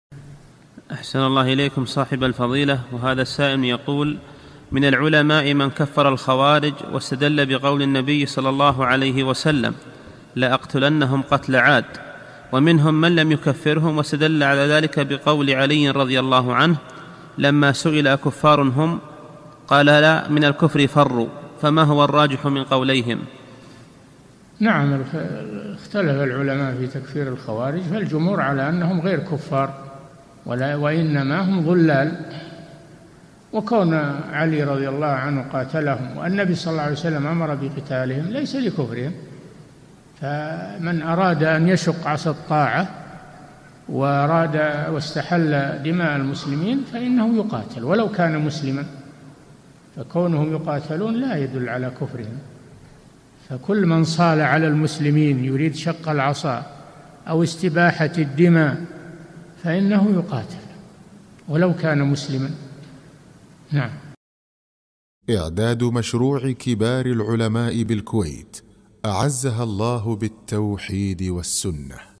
[359 -560] هل الخوارج كفار؟ - الشيخ صالح الفوزان
-"السؤال: من العلماء من كفر الخوارج واستدل بقول النبي ﷺ (لأقتلنَّهم قتل عاد)، ومنهم من لم يكفرهم واستدل على ذلك بقول عليّ رضي الله عنه لما سئل ""أكفار هم قال: لا، من الكفر فروا""، فما هو الراجح من قوليهم؟
الجواب: نعم اختلف العلماء في تكفير الخوارج، فالجمهور على أنهم غير كفار، وإنما هم ضلال، وكون عليّ رضي الله عنه قاتلهم، والنبي ﷺ أمر بقتالهم، ليس لكفرهم، فمن أراد أن يشق عصا الطاعة واستحل دماء المسلمين فإنه يقاتل ولو كان مسلما، فكونهم يقاتلون لا يدل على كفرهم، فكل من صال على المسلمين يريد شق العصا أو استباحة الدماء فإنه يقاتل، ولو كان مسلما.
للاشتراك في قناة الشيخ صالح الفوزان
سؤال وجواب من شرح السنة للبربهاري ( 560 ) مقطع

للاستفسار والإقتراحات والدعم
رئيس الفريق
لمشروع كبار العلماء